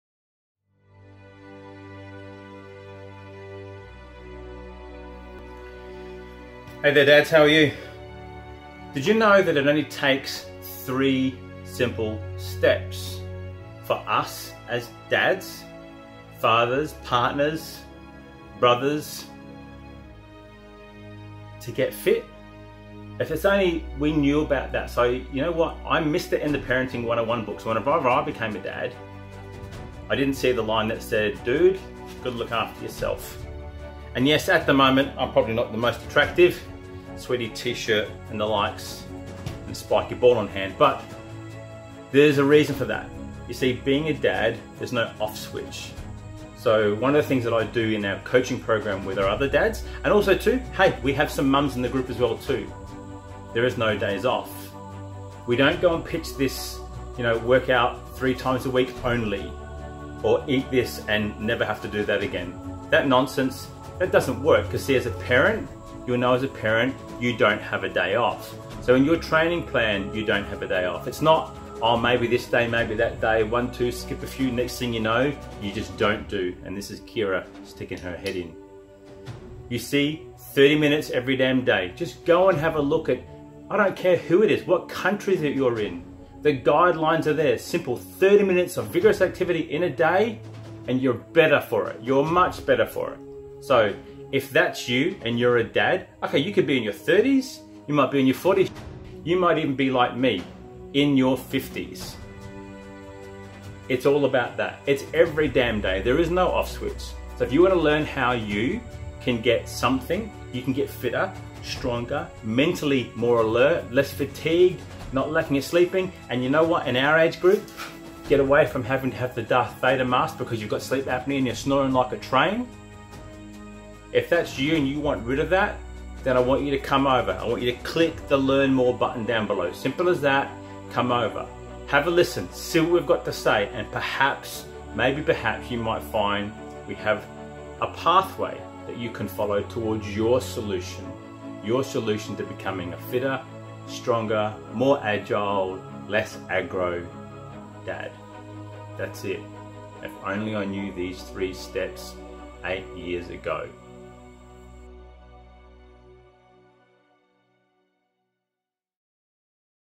3-Steps for Dad's to Drop 10 - 15 (1)
" DAD'S, THE 3 THINGS YOU MUST DO TO DROP THAT 10 - 15 KG AND KEEP IT OFF "

Watch this video ad learn how mastering the fundamentals using our 3-step system & coaching blueprint will get you results in 30-minutes a day!